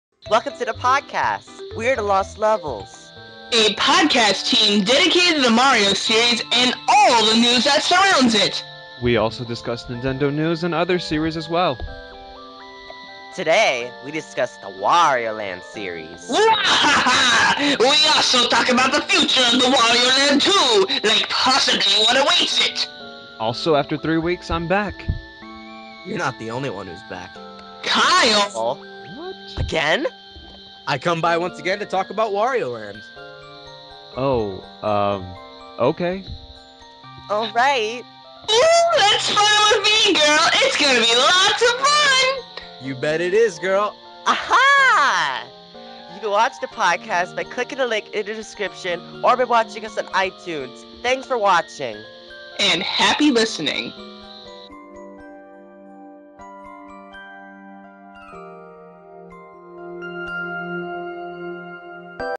Episode 58 - The Lost Levels
Today's podcast is about da' WARIO LAND series! We talk about Wario and his many antics in his well-known series, we also talk about the future of the Wario land too! Like what possibly awaits it! Kyle comes back to guest-star this week! SpiderStaryu hosts and his co-hosts are Toadette, SuperZambezi, and Kyle.